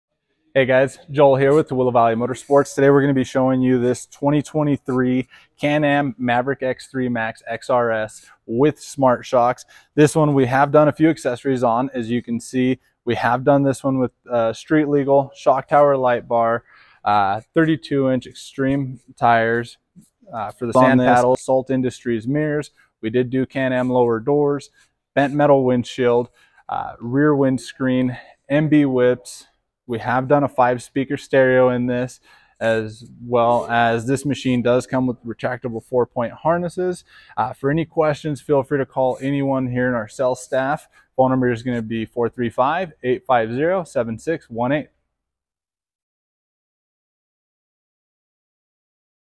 New X3 XRS Max Smart Shox floor build hits the showroom!
Take a look at a new X3 XRS Max Smart Shox floor build with our business manager Joel!